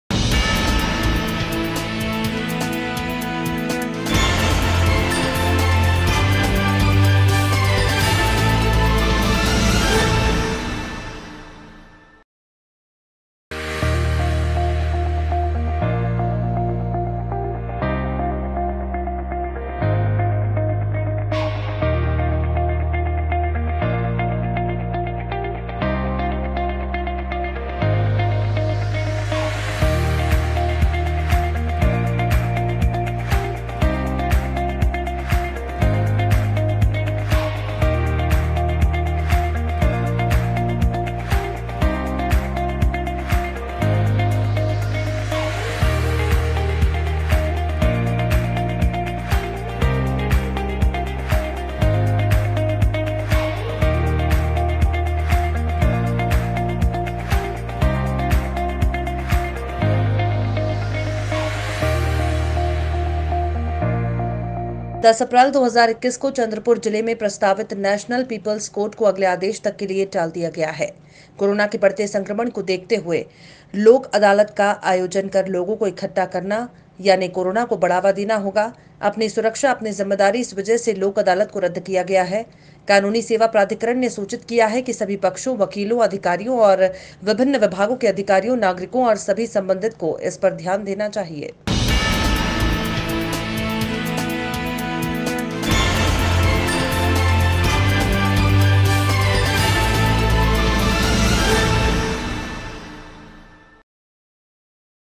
10 अप्रैल की लोक अदालत रद्द.......PARTHSARNEWS
10 अप्रैल, 2021 को चंद्रपुर जिले में प्रस्तावित नेशनल पीपल्स कोर्ट को अगले आदेश तक के लिए टाल दिया गया है। कोरोना के बढ़ते संक्रमण को देखते हुए, लोक अदालत का आयोजन कर लोगो को इकठ्ठा करना, याने कोरोना को बढ़ावा देना होगा। "अपनी सुरक्षा, अपनी जिम्मेदारी" इस वजह से लोक अदालत को रद्द किया है। कानूनी सेवा प्राधिकरण ने सूचित किया है कि सभी पक्षों, वकीलों, अधिकारियों और विभिन्न विभागों के अधिकारियों, नागरिकों और सभी संबंधितों को इस पर ध्यान देना चाहिए।